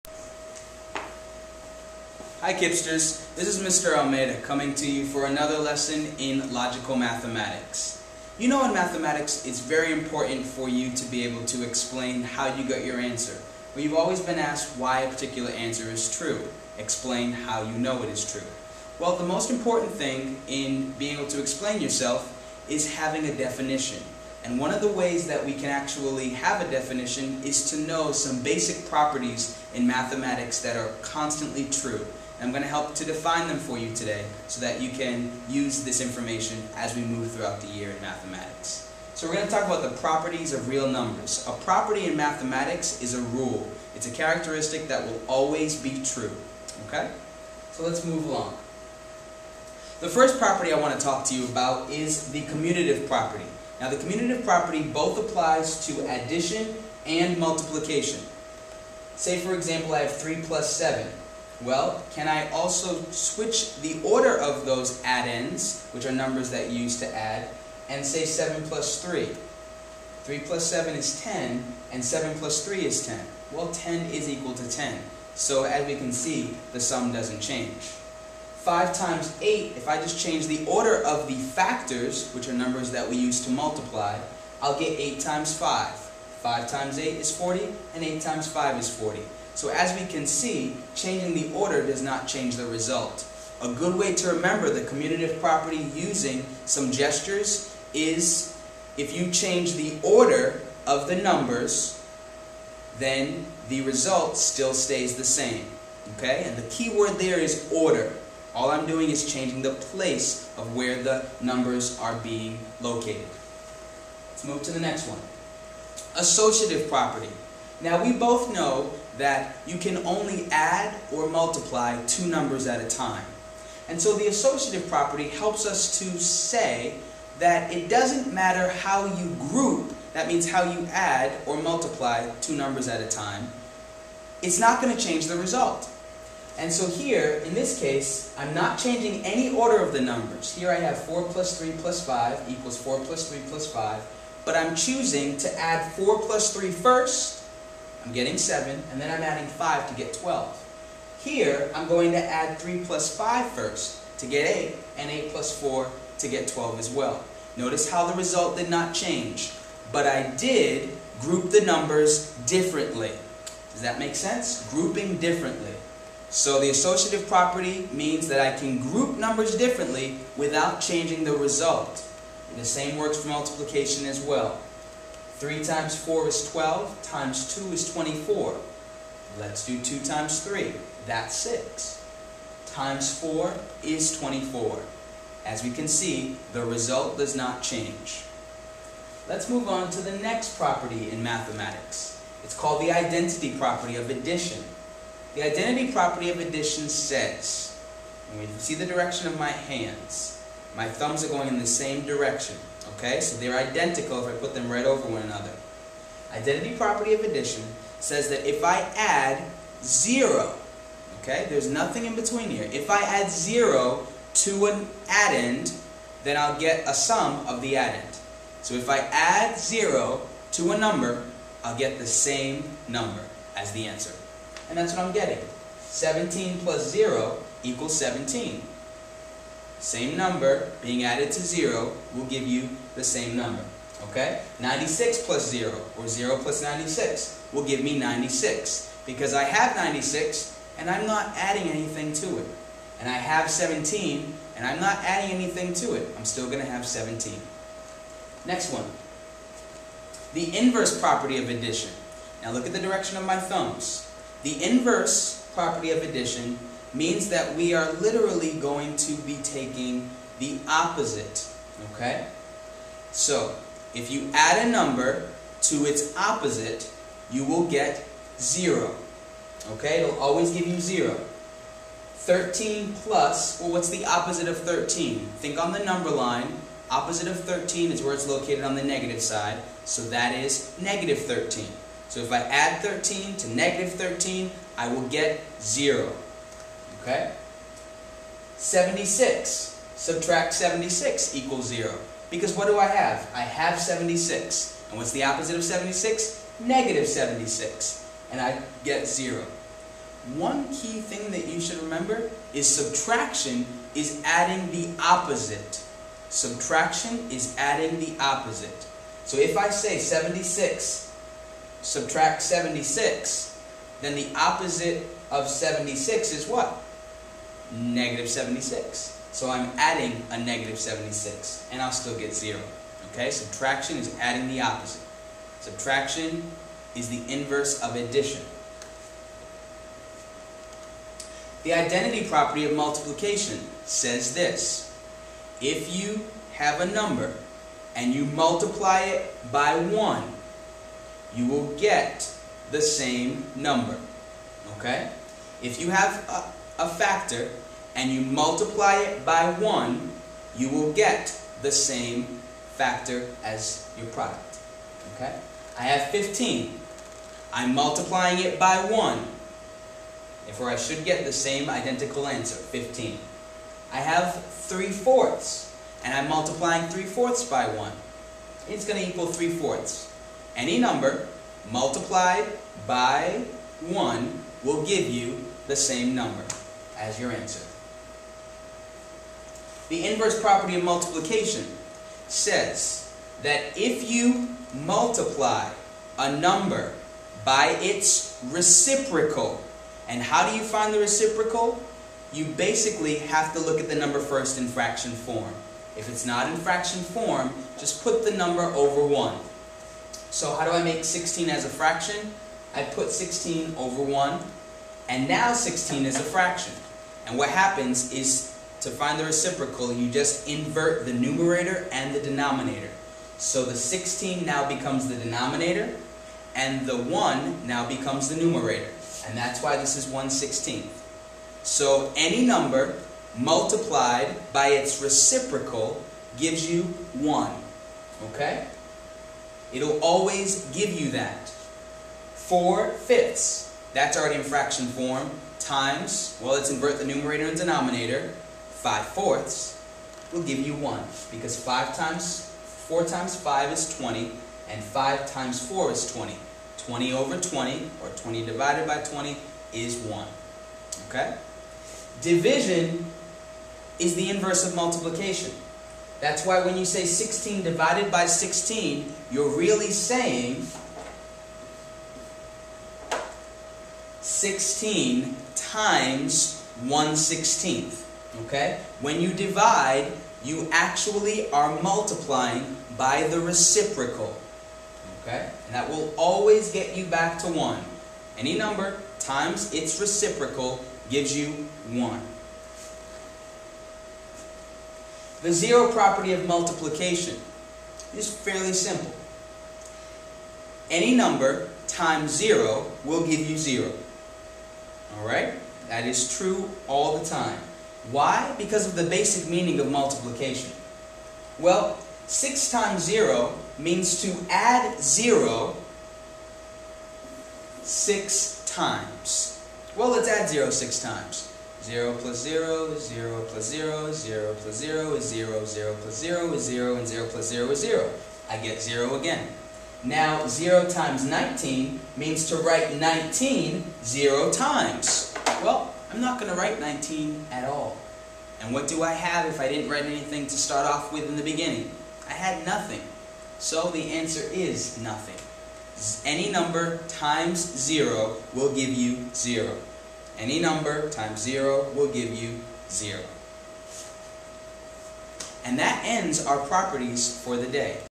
6.EE.3 / 7.EE.1 - Properties of Operations
In this video from Mr. Almeida, you will learn about the basic properties of real numbers in mathematics.  Ever wonder why 7 x 1 = 7?  Do you know what number multiplied by 16 gives you a product of 1?  You'll find the answers to these questions and more in this explanation of some of the properties in mathematics!  This meets all of the "properties of operations standards" in all the grades beginning in 2nd grade, but the properties become more meaningful when students begin to apply these properties to rewriting expressions (as in 6.EE.3 and 7.EE.1).